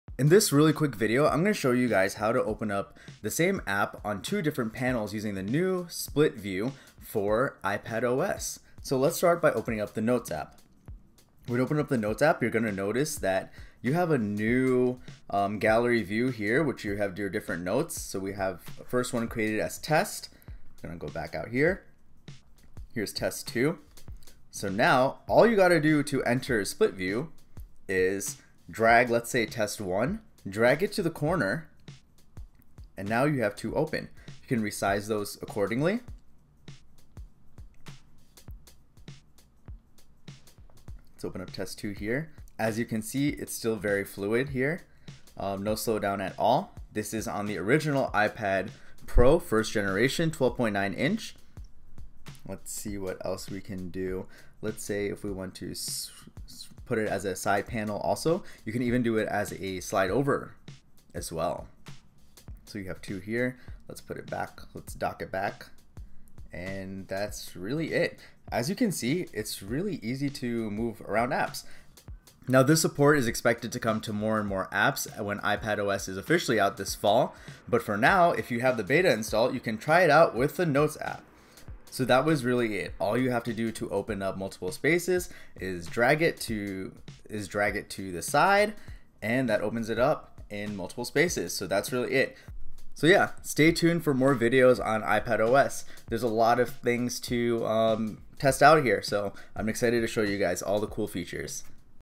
iPadOS | Apps in Multiple Spaces
Continuing with my exclusive in depth look at iPadOS, in this video I show you how the new operating system handles the same app in multiple spaces!